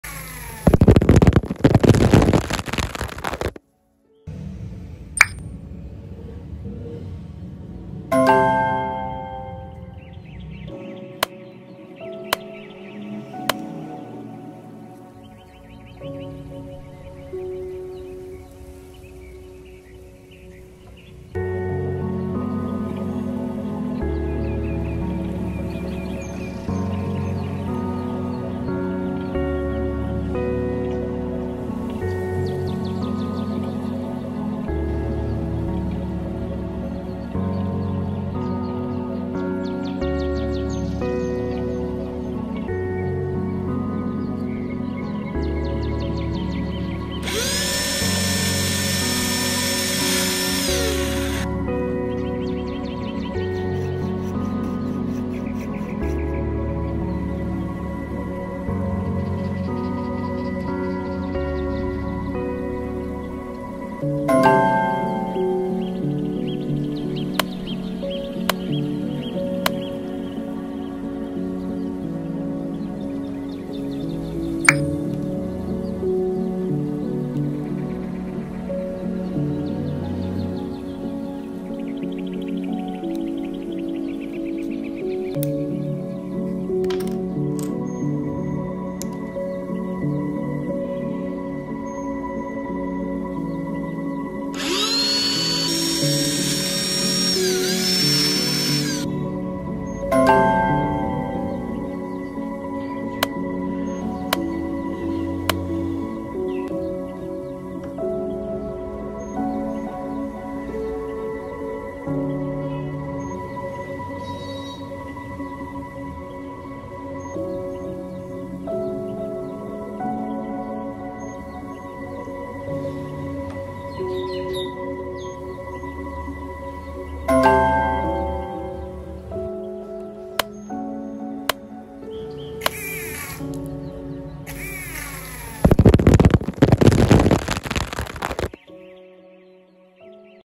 High pressure water pump
How to make high pressure water pump

Amazing water pump
Cool water pump
Dc motor water pump
Water pump